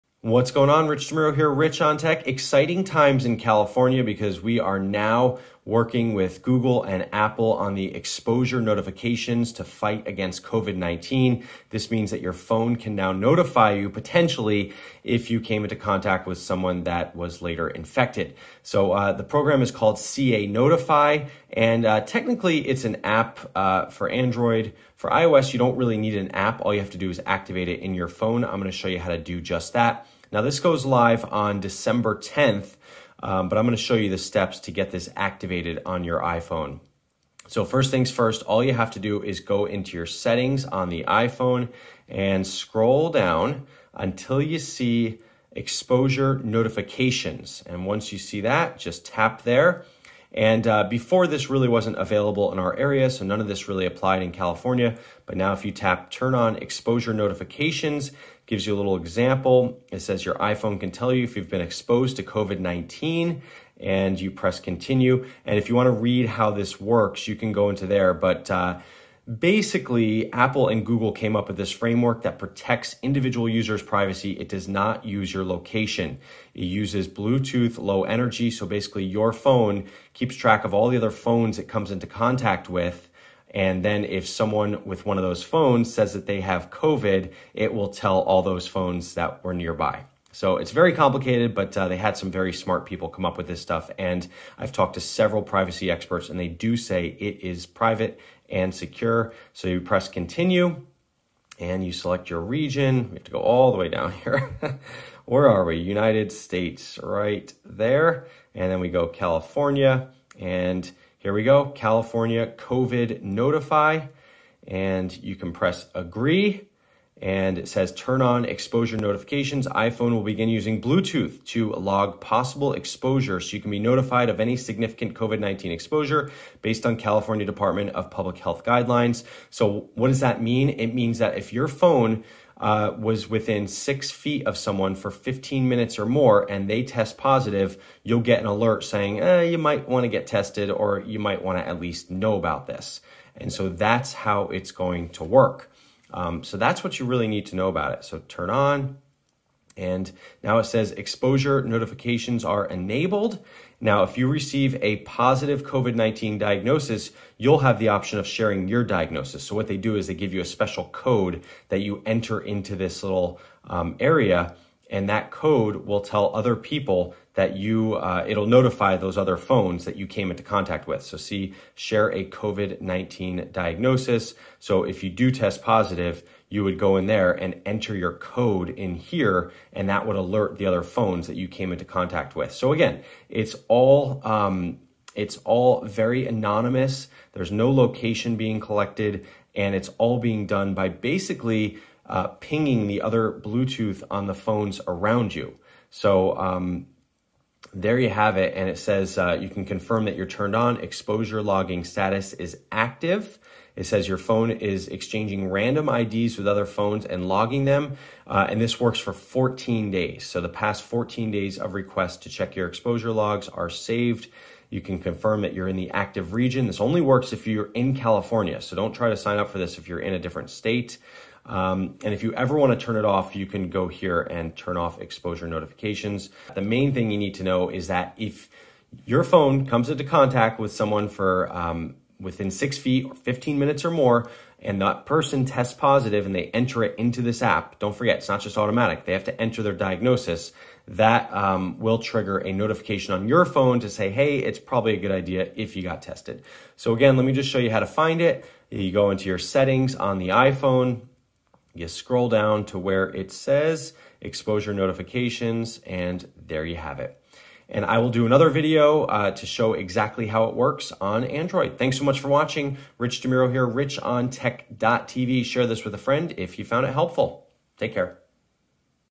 How to turn on California’s COVID-19 exposure notifications on iPhone
CA Notify will be available statewide starting December 10th on iOS 13.7 or later. You can activate it on the iPhone with no app download necessary.

1. Go into settings
2. Scroll down to Exposure Notifications
3. Turn on Exposure Notifications
4. Select United States, California